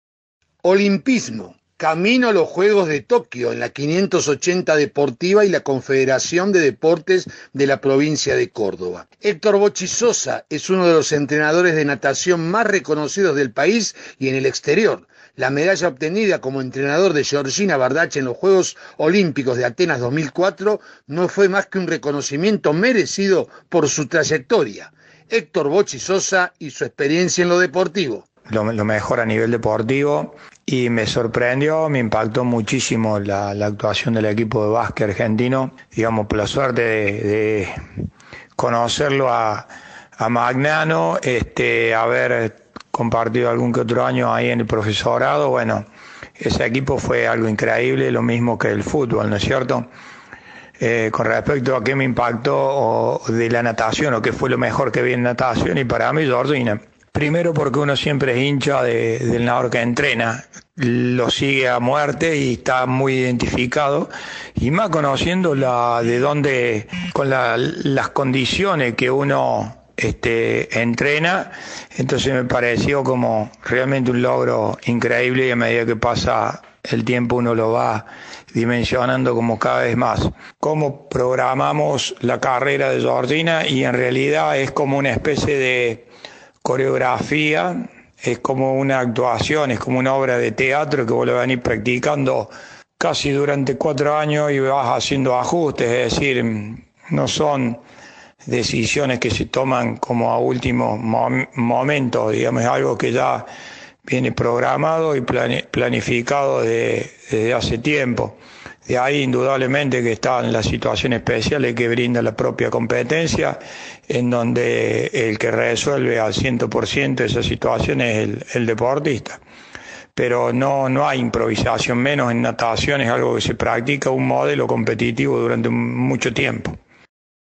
LA 580 DEPORTIVA Y LA CONFEDERACIÓN DE DEPORTES
EL OLIMPISMO, HÉCTOR "BOCHI" SOSA Y SU EXPERIENCIA EN LO DEPORTIVO.